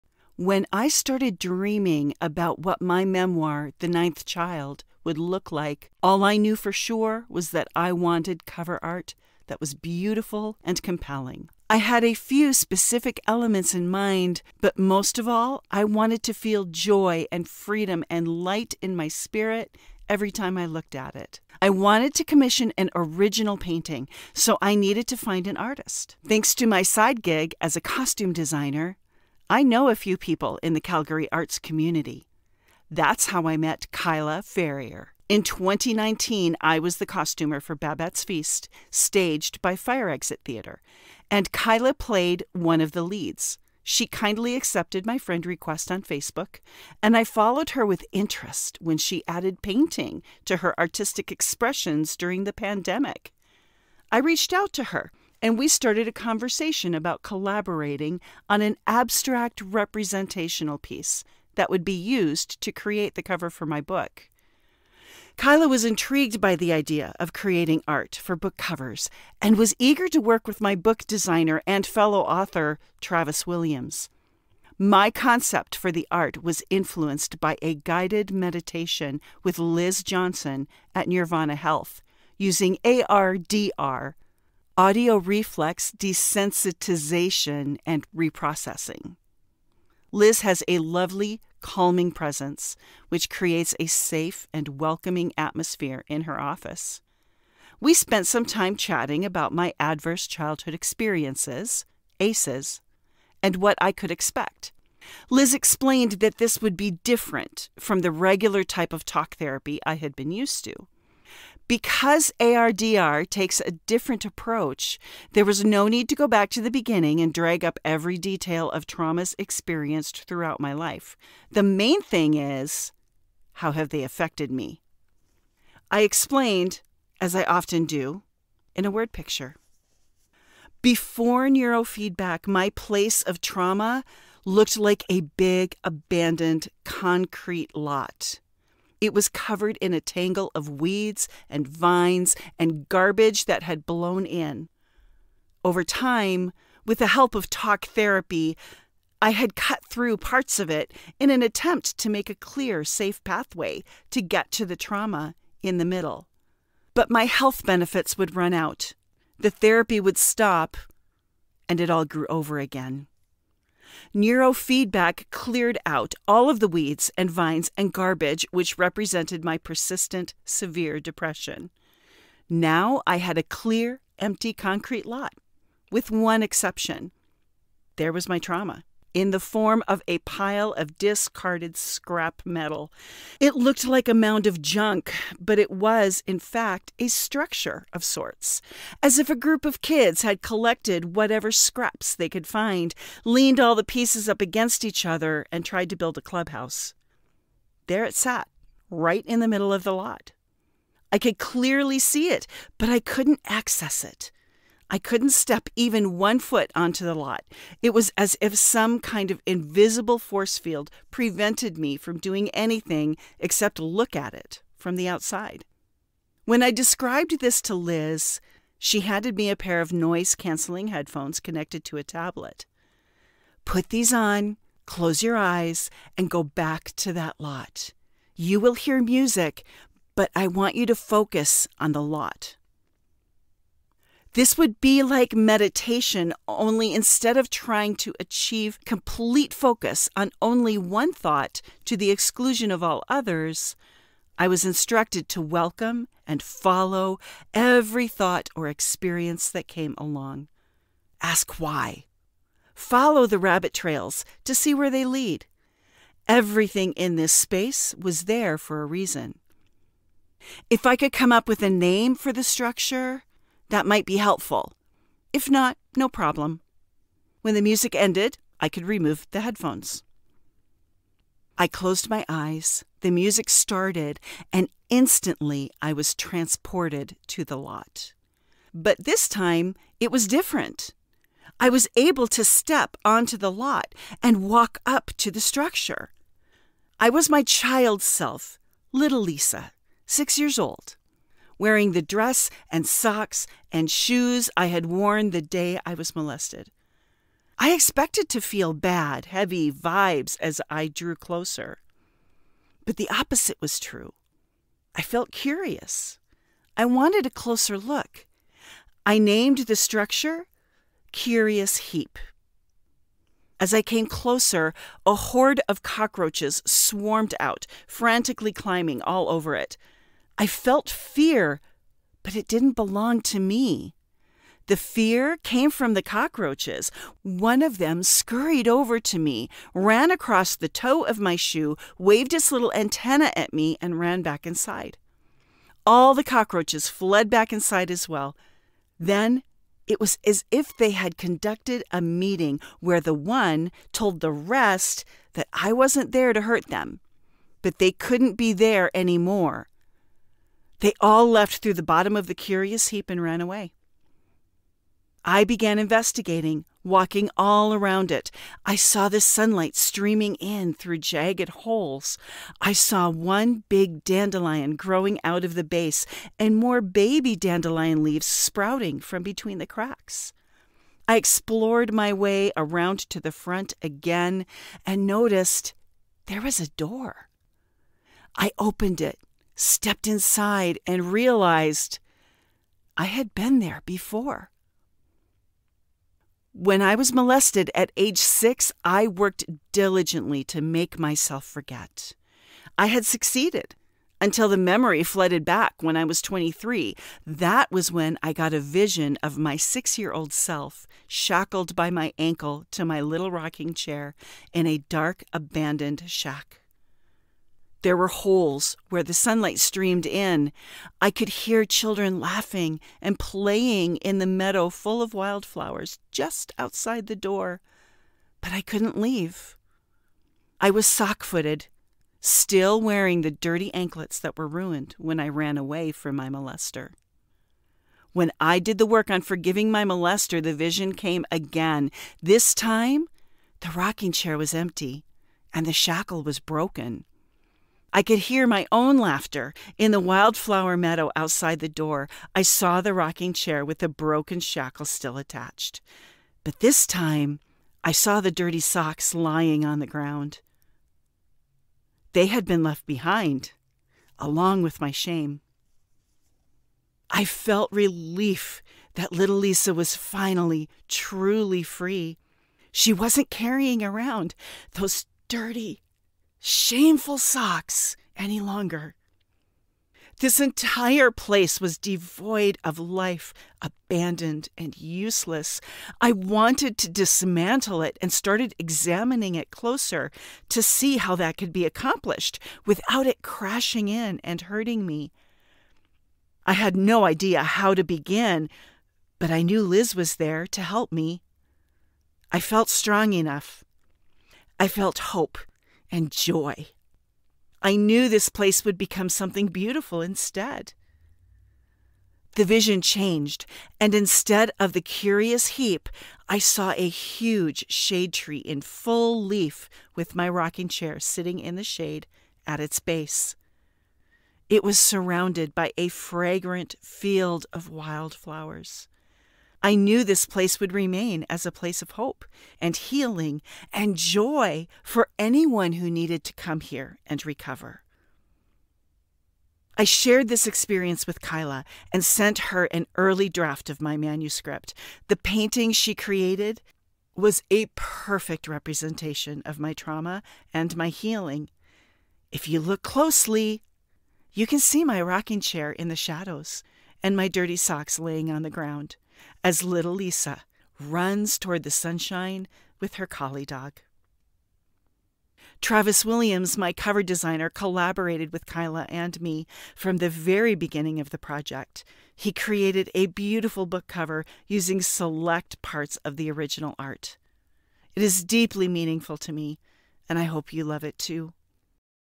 Journey to Cover Art - The Ninth Child by Lisa May LeBlanc
May is Mental Health Awareness month, and Lisa May LeBlanc is leaning into the conversation with the audiobook of her memoir, The Ninth Child.

Lisa May LeBlanc's memoir grapples with some heavy themes regarding childhood trauma and her adult journey to healing, alongside some more lighthearted observations about growing up on a farm

The original cover art for Lisa's book captures all of this and more.

Excerpt from Lisa's audiobook.